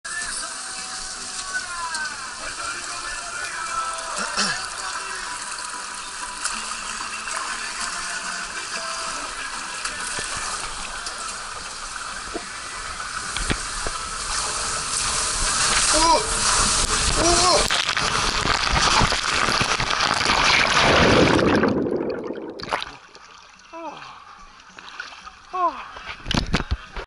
Twister Water Slide at Albercas El Vergel
Twister water park slide at Albercas El Vergel aqua park in Tijuana, Mexico.